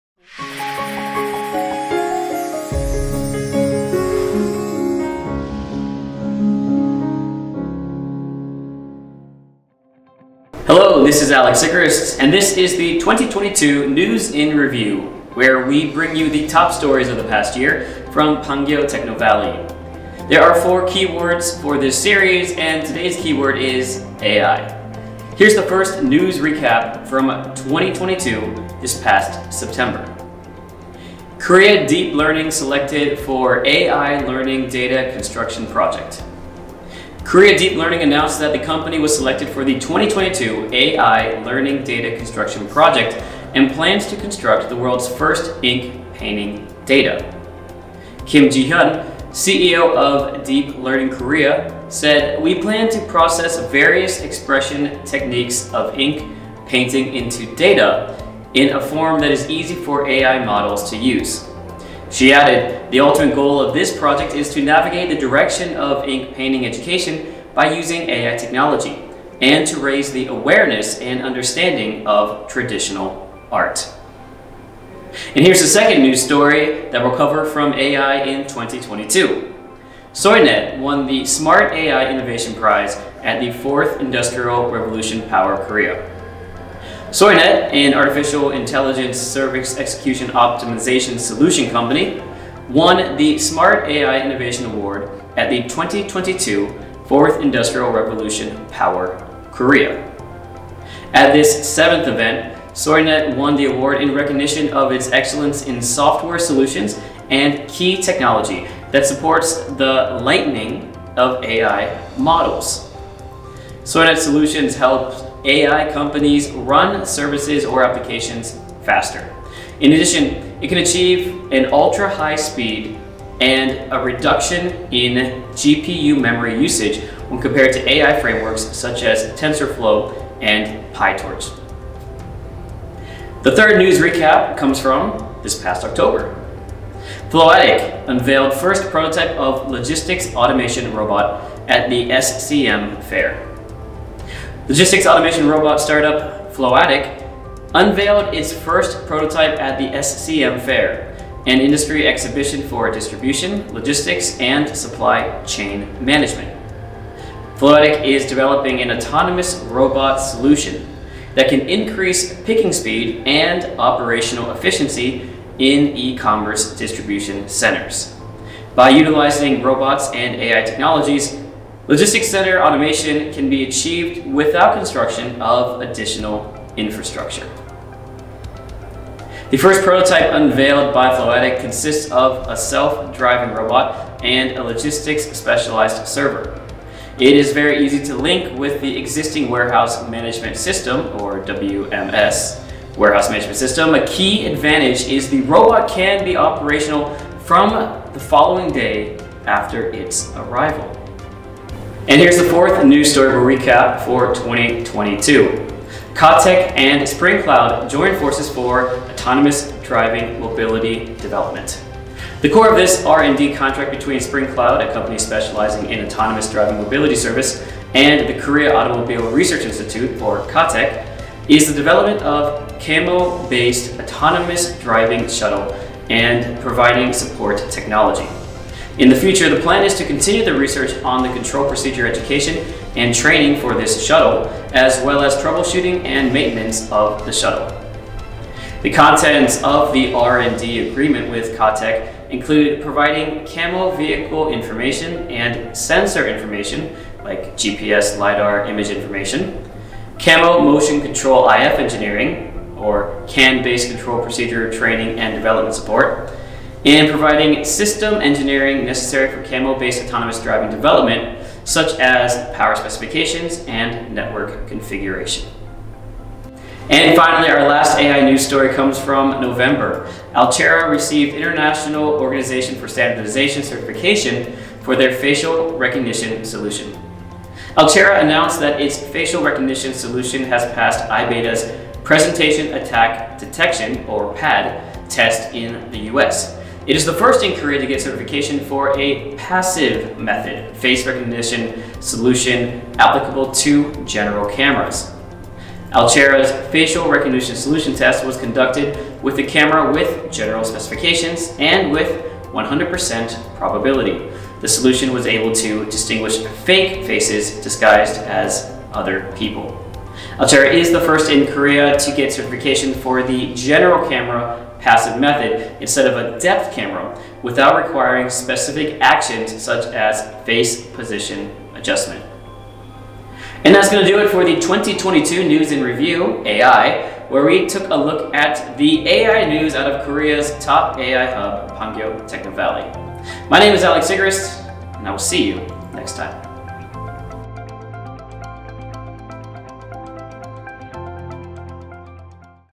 Pangyo Technovalley, 2022 News in Review : AI
📺  Pangyo Techno Valley Weekly news

This is the ‘2022 News in Review’ where we bring you the top stories of the past year from Pangyo Techno Valley. There are four keywords for this series, and today’s keyword is “AI.”

Here’s the first news recap from 2022 this past September.
1. Korea Deep Learning selected for AI learning data construction project.
2. Soynet won the prize called "Smart AI Innovation" at the Forth Industrial Revolution Power Korea.
3. Floatic unveils first prototype of ‘logistics automation robot’ at the SCM Fair.
4. Spring Cloud and Korea Automotive Technology Institute (Katech) join forces for research and the development of       autonomous driving technology support business!
5. Alchera Got Their Facial Recognition Solution ‘International Organization for Standardization’ Certification.